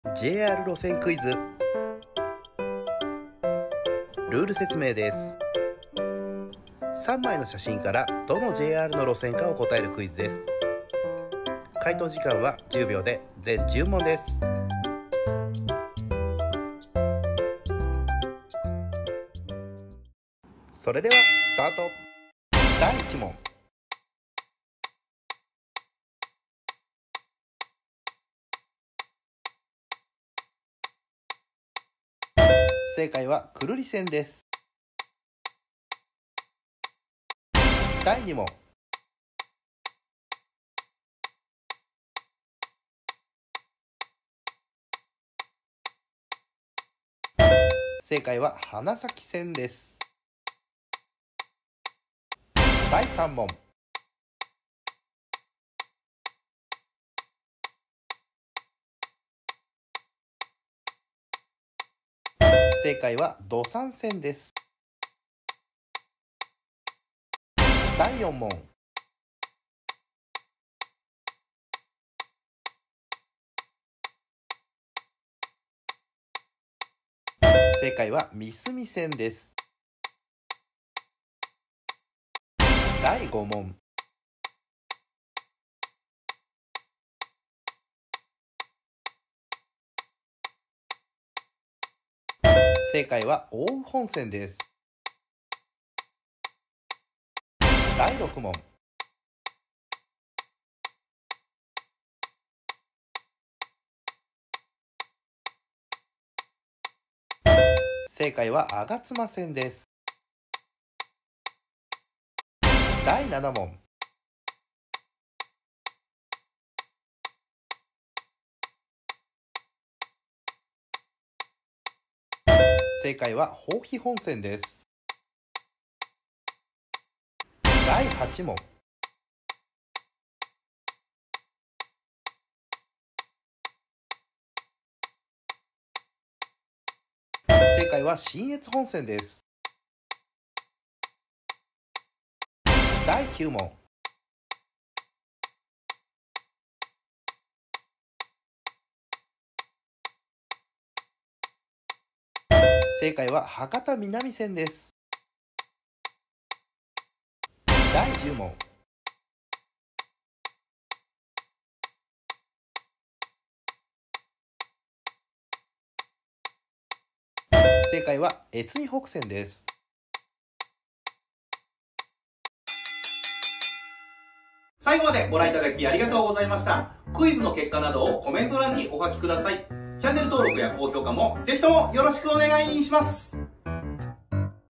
【鉄道クイズ】 路線の写真クイズ
３枚の写真からどのJR路線か答えるクイズです

回答時間は1問10秒で全10問です。

鉄道大好き芸人の太田トラベルのチャンネル

このチャンネルでは私の鉄道ネタや鉄道動画や鉄道クイズなどを運行(アップロード)しております

SNSも運行中です！